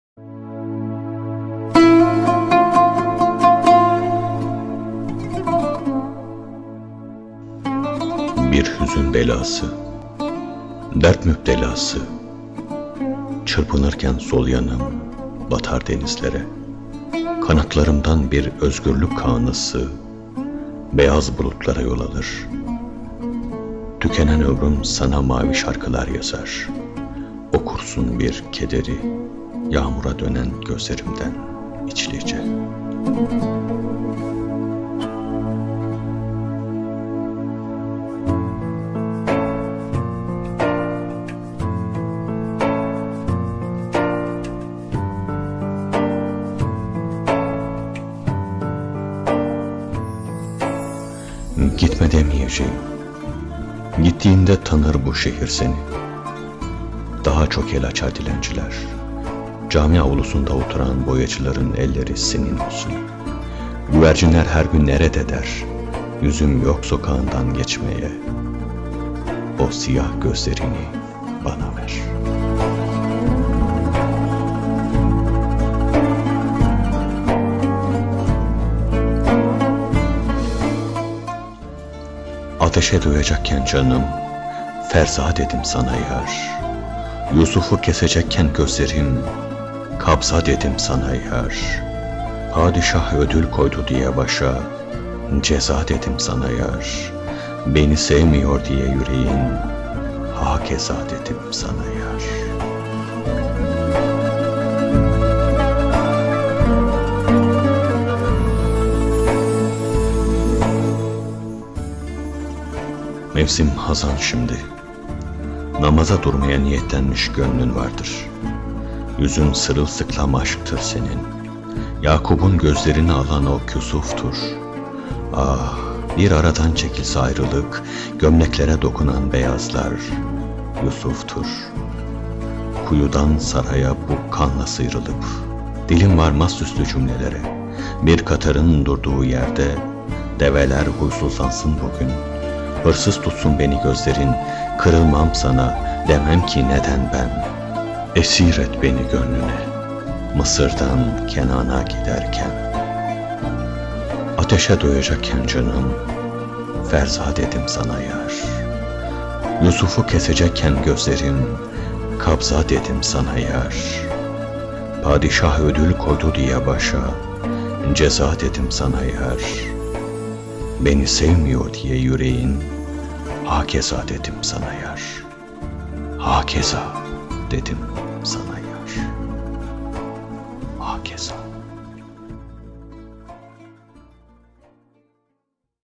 Bir Hüzün Belası - Ahmet ORMANCI
''Bir Hüzün Belası'' Muhteşem Bir Hatice EKİN Şiiri,  Ahmet ORMANCI Yorumuyla...